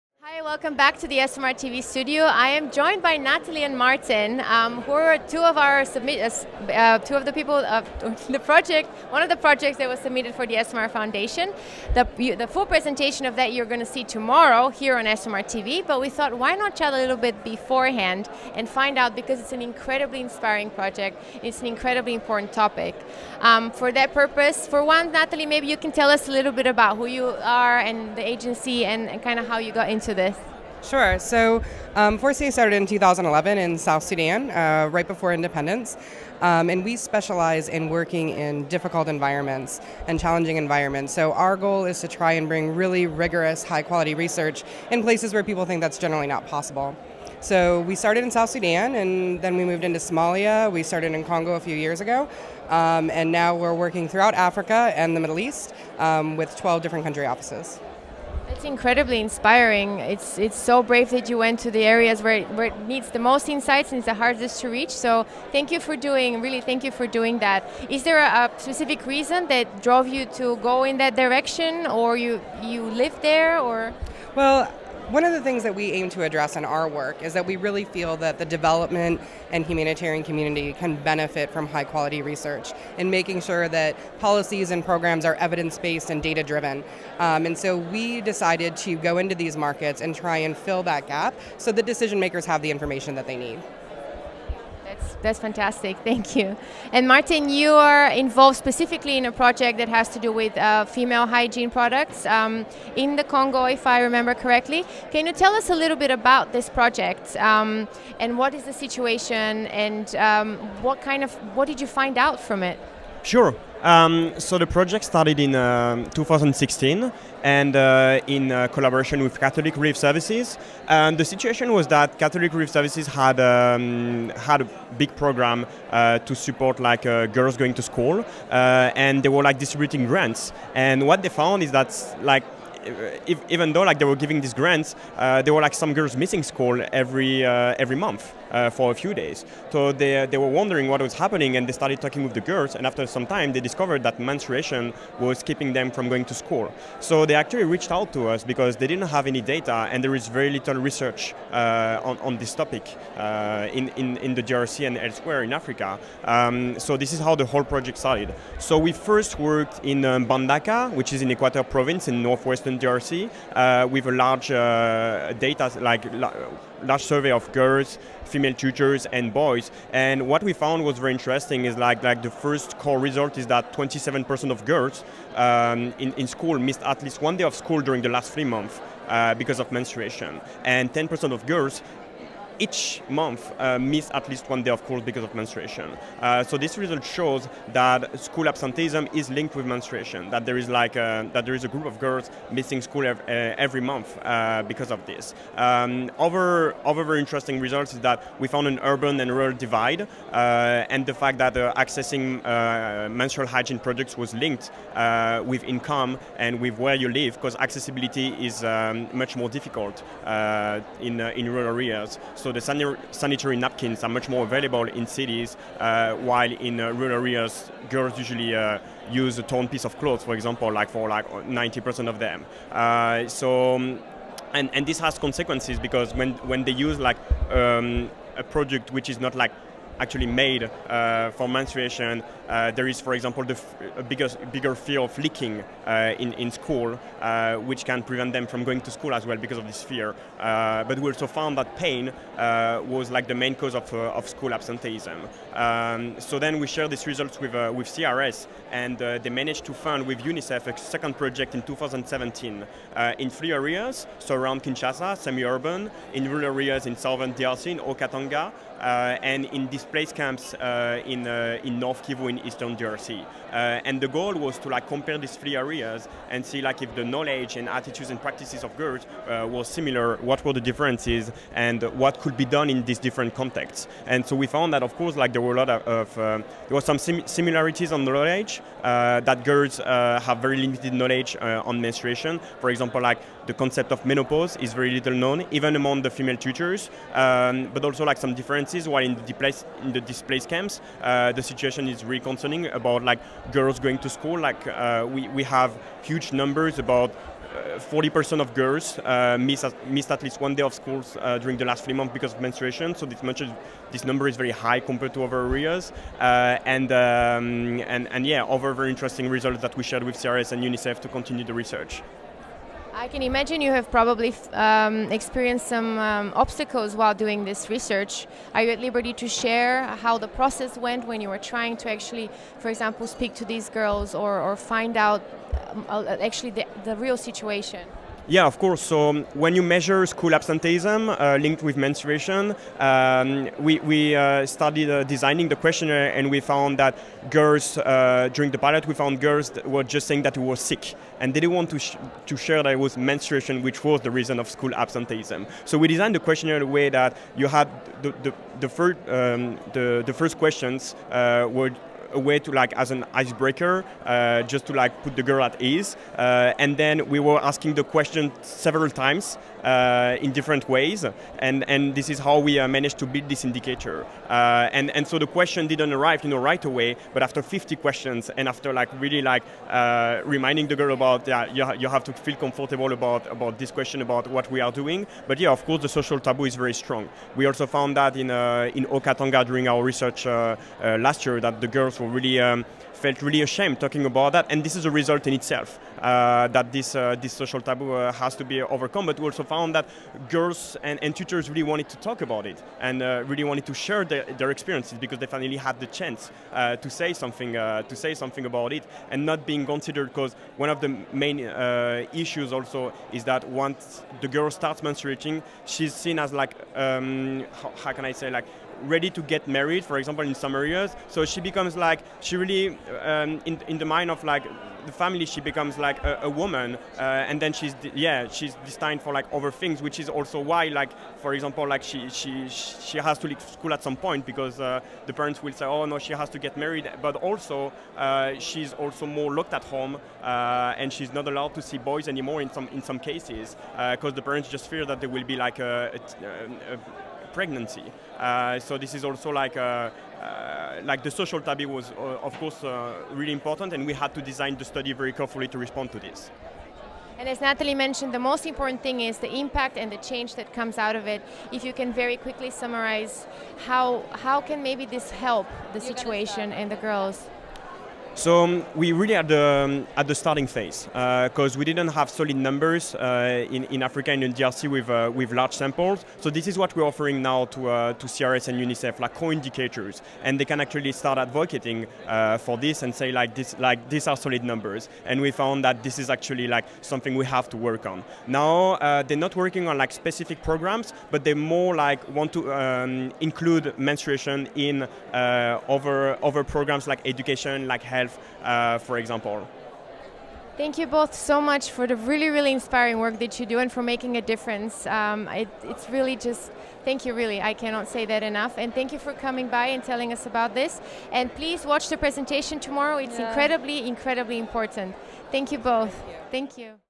Making a Difference Competition winners 2018 - Forcier Consulting
ESOMAR Congress 2018 interview with Natalie Forcier and Martin Kern. Winners of the Best local/domestic NFP case study of our Making a Difference Competition with the "Menstrual Hygiene Management Study in D.R.Congo". This project was carried out by Forcier Consulting on behalf of Catholic Relief Services D.R. Congo.